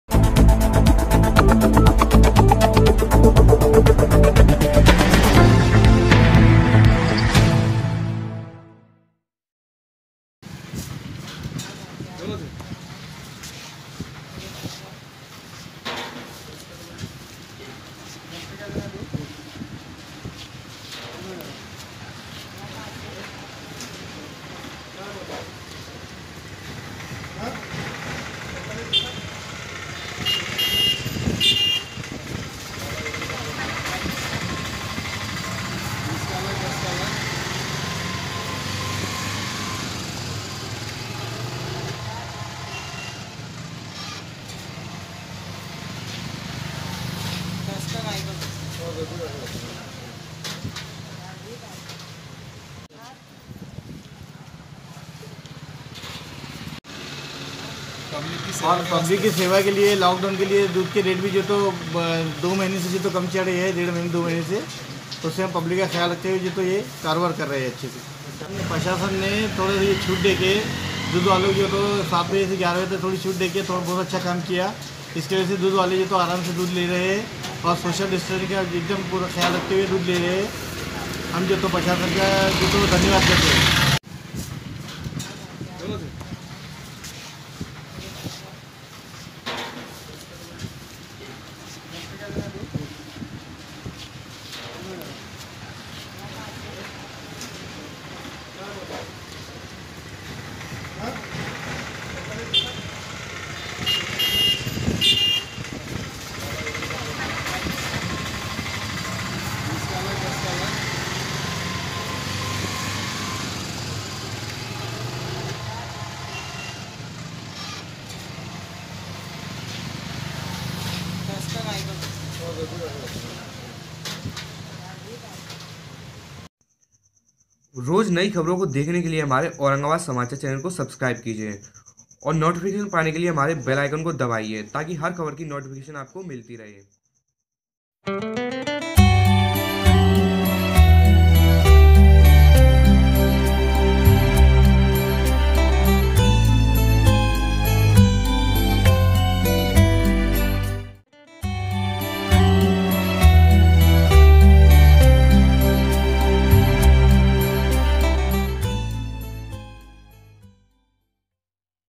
औरंगाबाद मे दूध डेरी खोलने के फैसले से नागरिकों को राहत दूध डेरीयों पर गर्दी कम
लाँकडाउन के कारण दूध के भाव मे विषेश छूट सोशल डिस्टेंसिंग मे जारी दूध वितरण